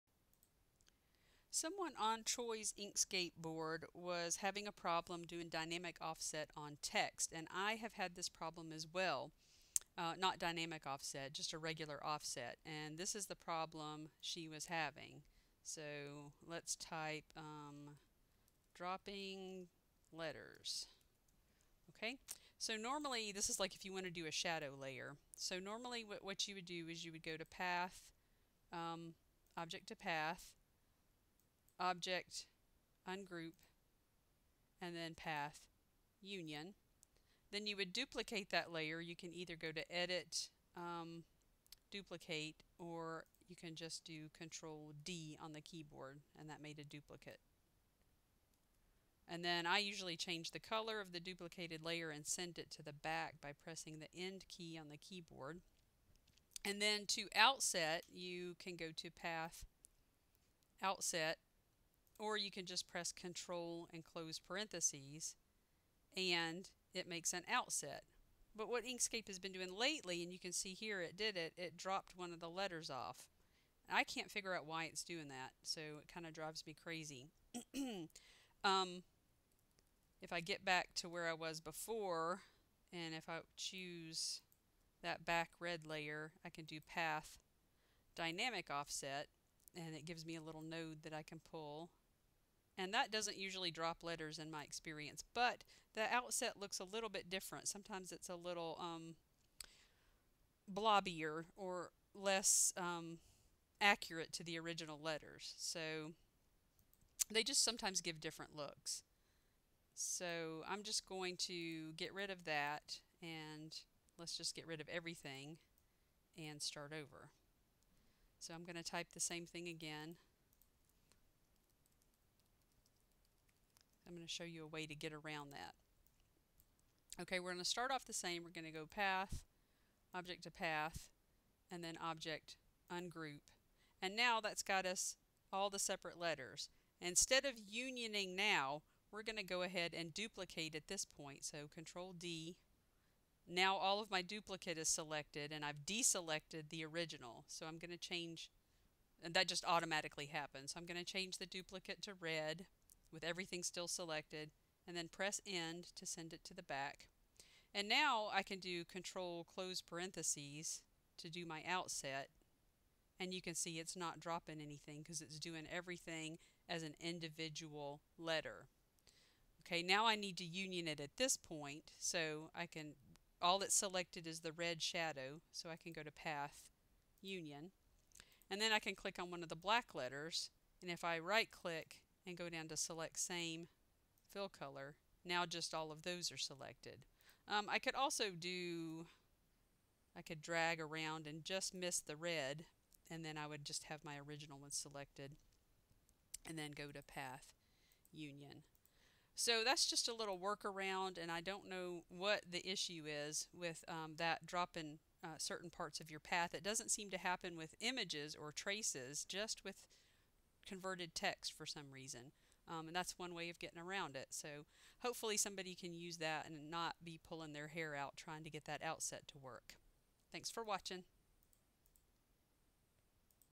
Inkscape- bug workaround, outset dropping letters
Update: Uninstalling inkscape and reinstalling solved this issue, so I don't know what the deal was. It wasn't anything with my default settings, because I saved those and copied them into the new installation.  I'll leave this up in case someone needs a quick work around with no time to reinstall.

Inkscape seems to have a bug that crops up when trying to do an outset or shadow layer on text that has been converted to a path.  This is a work around for that problem. Dynamic offset is another way to do it that doesn't seem to drop letters, but sometimes that does not look quite the same as outset.